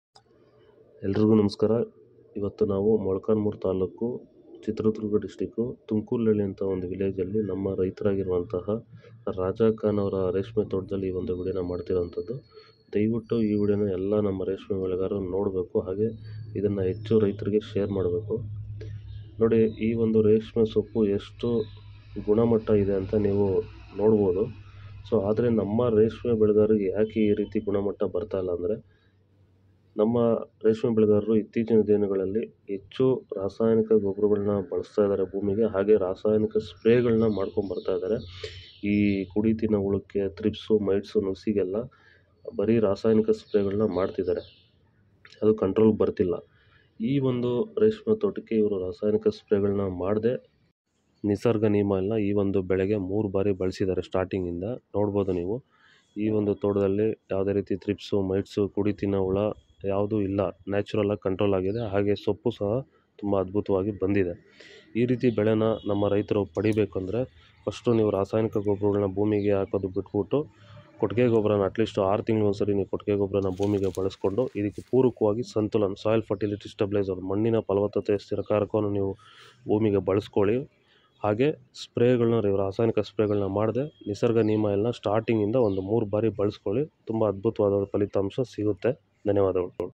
ಗುಣಮಟ್ಟದ ರೇಷ್ಮೆ ಬೆಳೆ ಬೆಳೆಯಲು ಭೂಮಿಗೆ ಸಂತುಲನ್ ಹಾಗೂ ನಿಸರ್ಗ Neem Oil Spary ಮಾಡಿ | SANTOOLAN | Mulberry
ಗುಣಮಟ್ಟದ ರೇಷ್ಮೆ ಬೆಳೆ ಬೆಳೆಯಲು ಭೂಮಿಗೆ ಸಂತುಲನ್
ಹಾಗೂ ನಿಸರ್ಗ Neem Oil Spary ಮಾಡಿ.

ಭೂಮಿಯ ಫಲವತ್ತತೆಯನ್ನು ಹೆಚ್ಚಿಸಿ, ಗುಣಮಟ್ಟದ ಬೆಳೆಯನ್ನು ಬೆಳೆಯಲು ಭೂಮಿಗೆ ಸಂತುಲನ್ ನನ್ನು ಬಳಸಿ.

"ಶ್ರೀ ಪರಿಸರಮ್ಮ ಅಗ್ರೀ ಸಲ್ಯೂಷನ್" ನಮ್ಮನ್ನು ಸಂಪರ್ಕಿಸಿದ್ದಕ್ಕೆ ನಿಮ್ಮಗೆ  ಧನ್ಯವಾದಗಳು.. ಹೆಚ್ಚಿನ ಮಾಹಿತಿಗಾಗಿ ಕರೆ ಮಾಡಿ-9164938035

*Sri Parisaramma Agri Solution - ಶ್ರೀ ಪರಿಸರಮ್ಮ ಅಗ್ರೀ ಸಲ್ಯೂಷನ್*